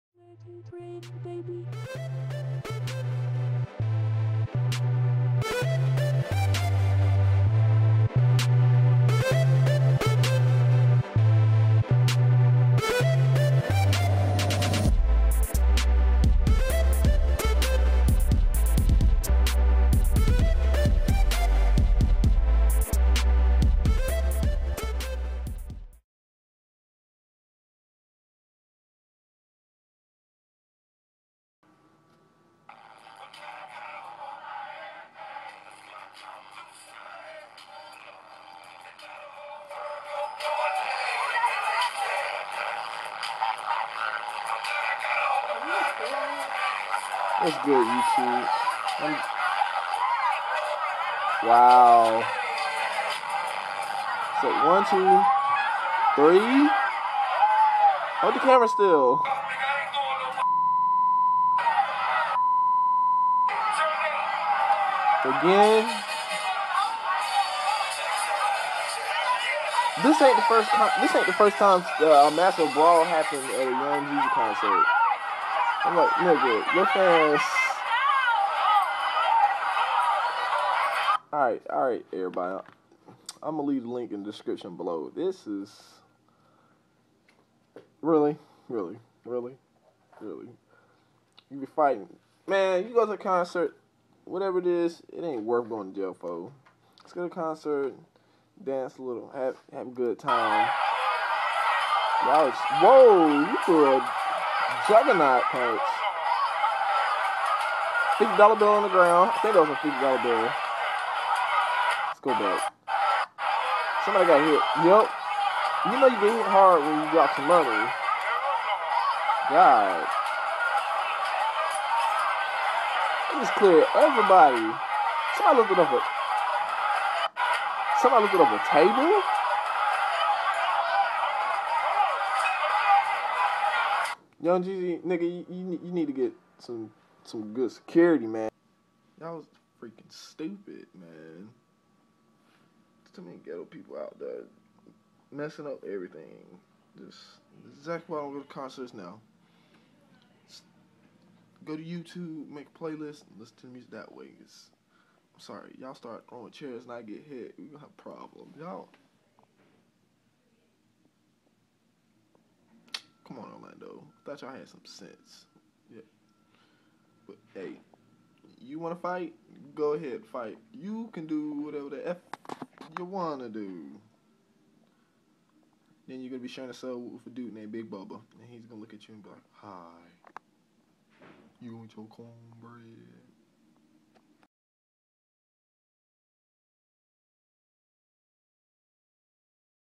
Young Jeezy Concert Brawl
My first reaction to seeing a TMZ video where a mass of ppl begin to duke it out.